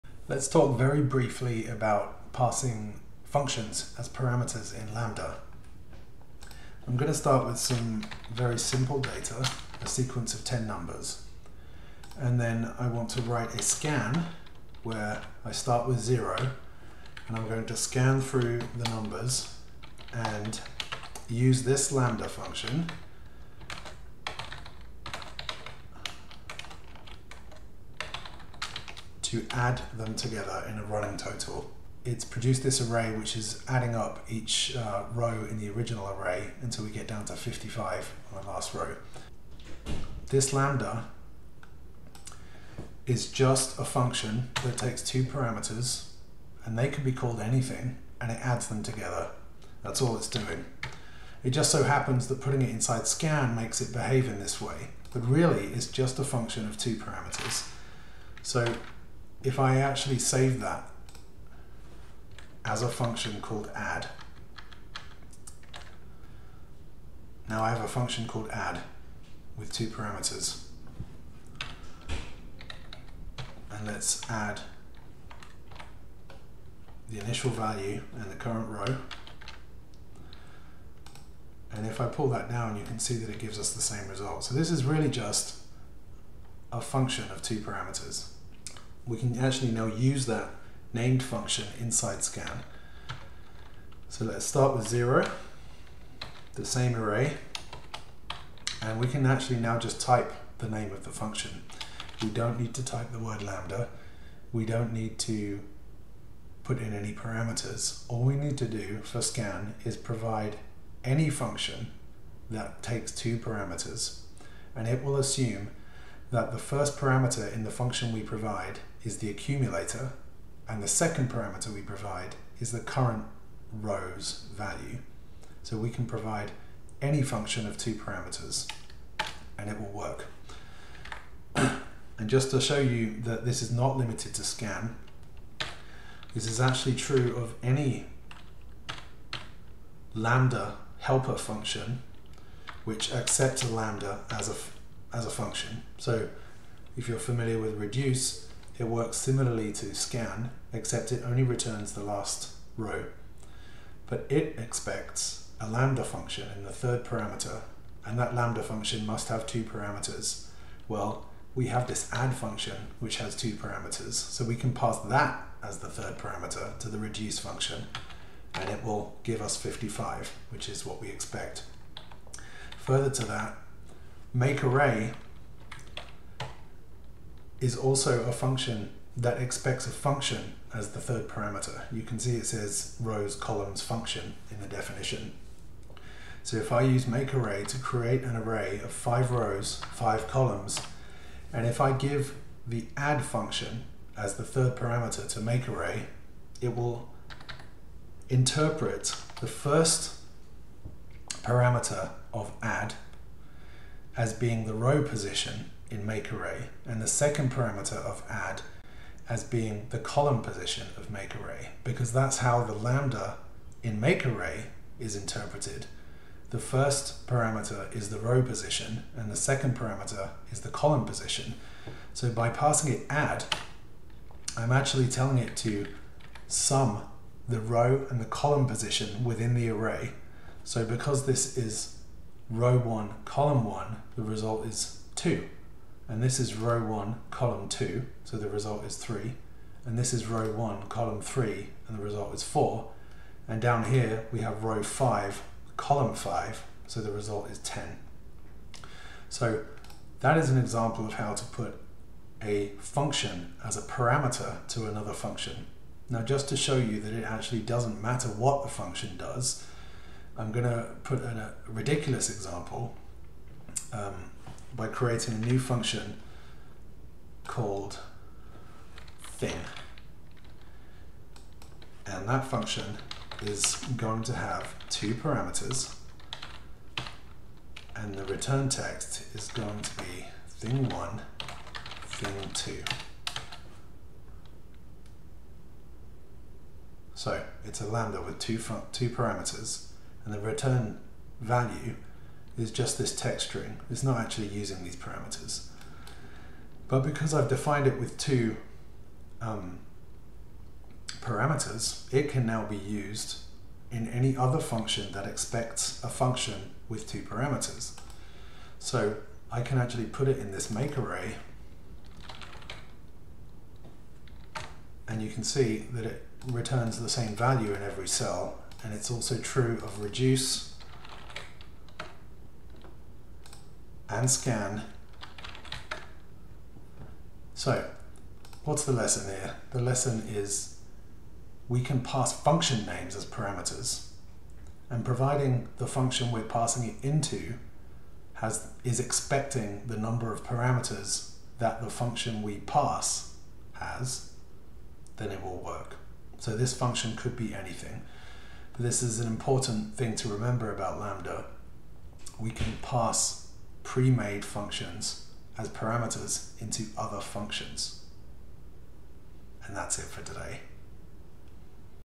Functions as parameters with Excel LAMBDA
Provides a brief introduction to pass functions as parameters to other functions.

This foundational concept will greatly help you modularize your lambda developments in Excel and yield significant performance improvements.